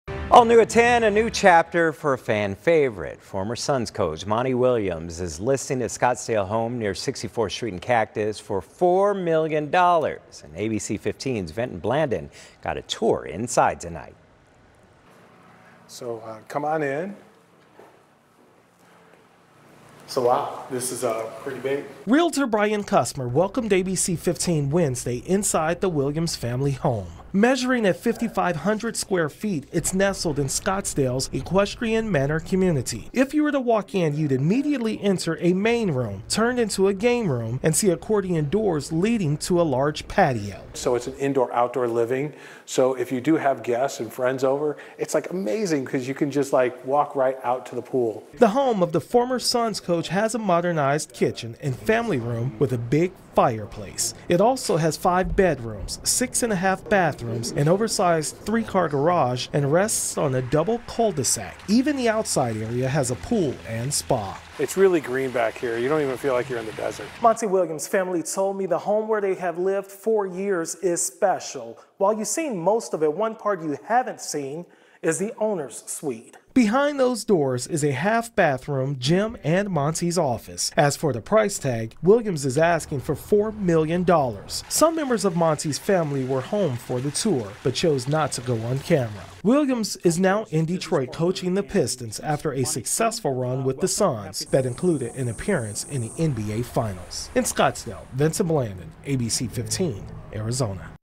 Monty Williams' home listed for $4 million
As Monty Williams prepares to start his role as head coach in Detroit, he has listed his home for $4 million.